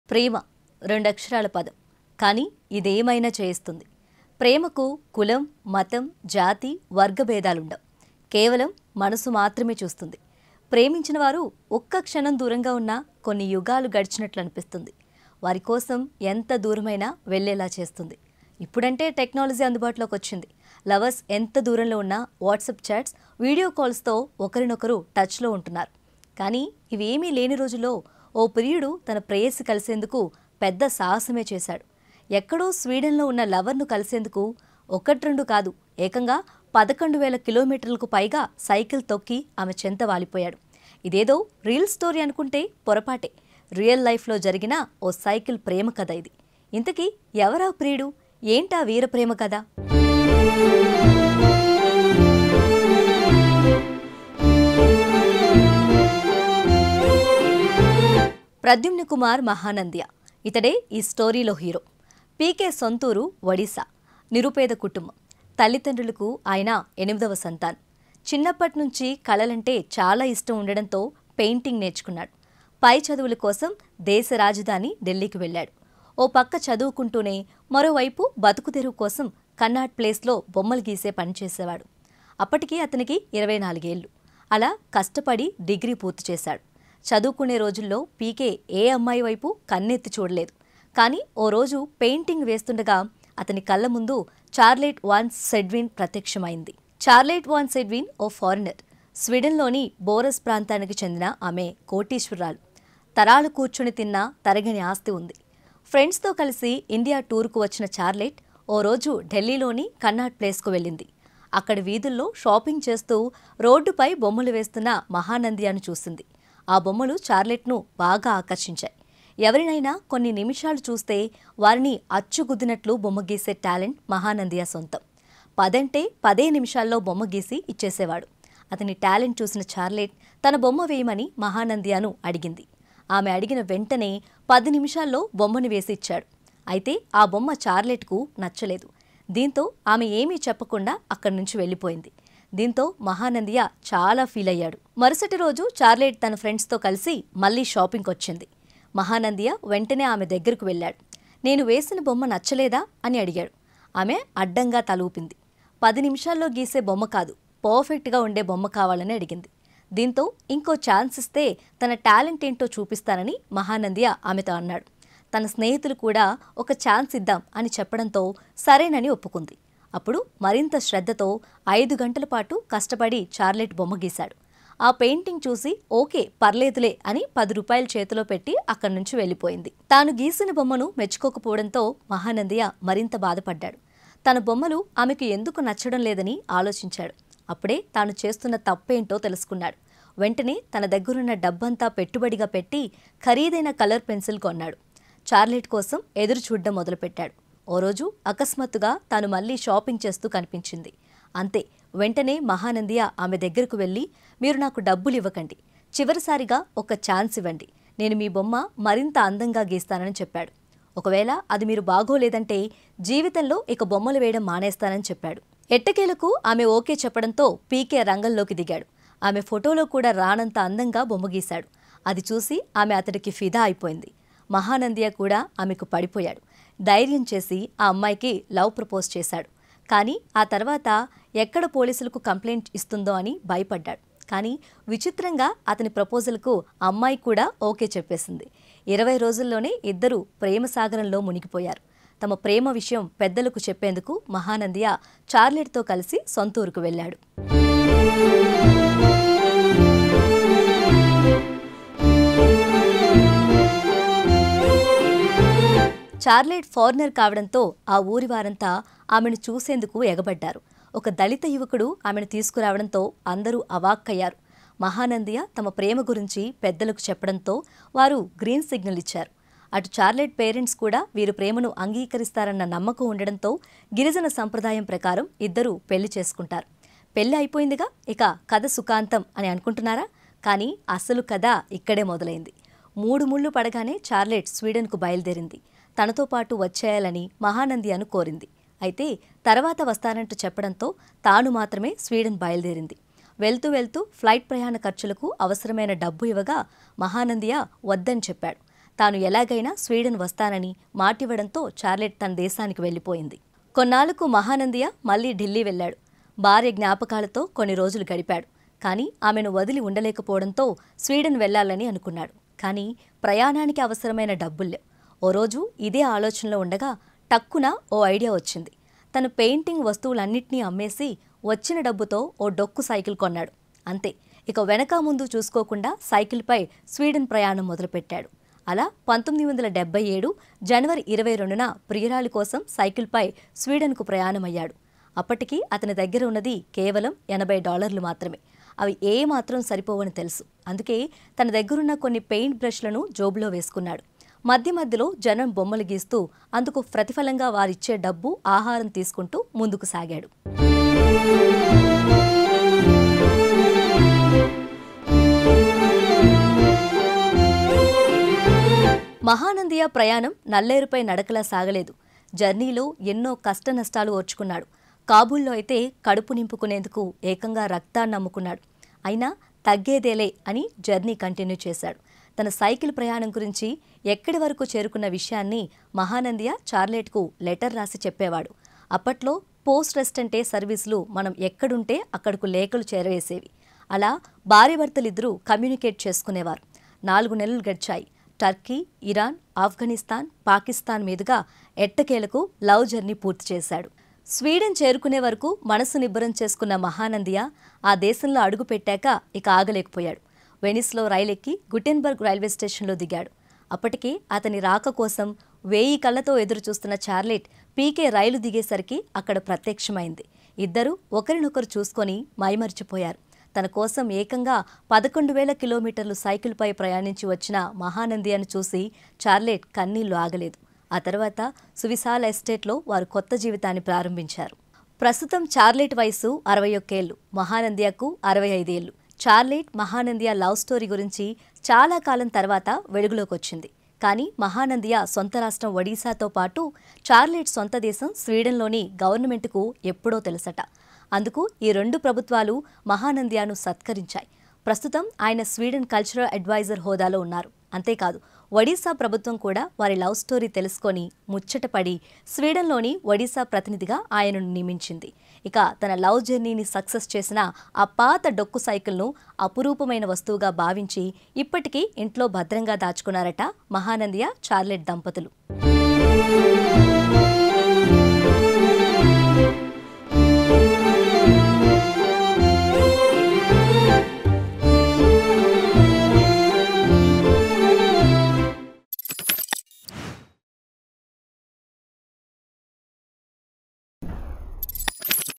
Epic Bicycle Love Story: PK Mahanandia's Journey from India to Sweden for Love | Mic TV News
The incredible true love story of PK Mahanandia, who embarked on an epic bicycle journey from India to Sweden to reunite with his beloved Charlotte van Sedwin. This heartwarming tale of love, determination, and adventure will leave you inspired. From humble beginnings in Odisha to overcoming immense challenges, PK's 11,000 km journey is a testament to the power of love. Learn about their unique romance, the trials they faced, and how their story continues to inspire people around the world. Watch now to follow their unforgettable journey!